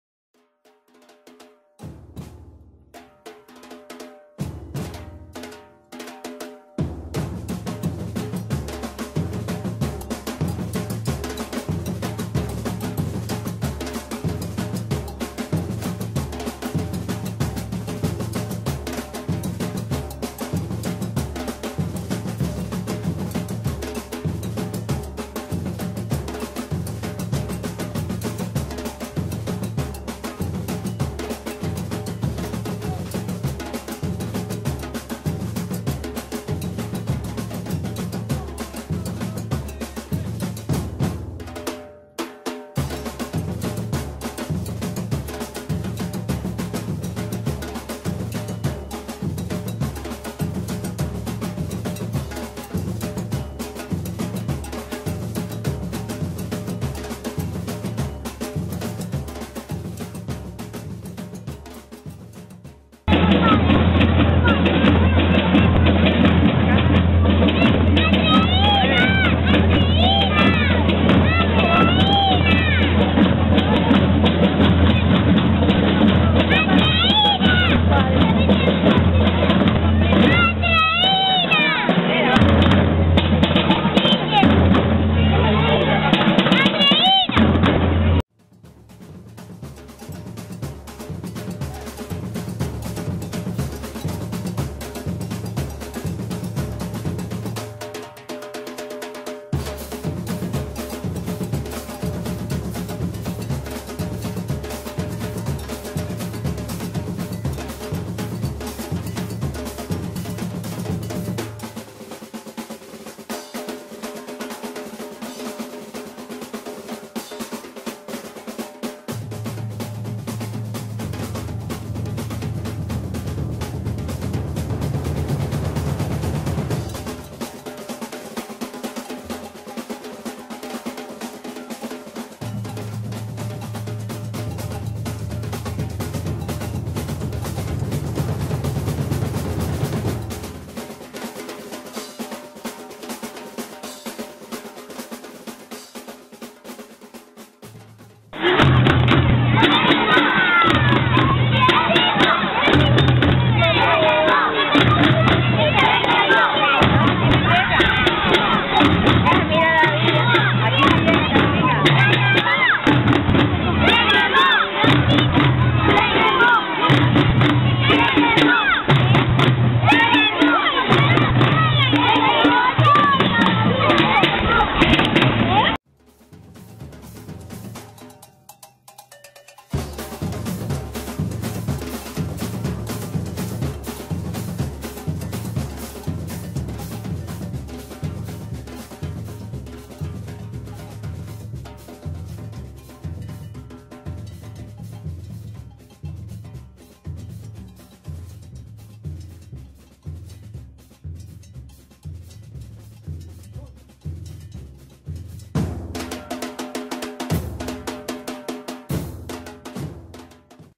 Entierro de la Sardina 2012 - Ceip Maximiliano Gil
Entierro de la Sardina celebrado en el CEIP Maximiliano Gil (Tacoronte-Tenerife). La sardina fue acompañada musicalmente por una batucada interpretada por alumnado de 6º.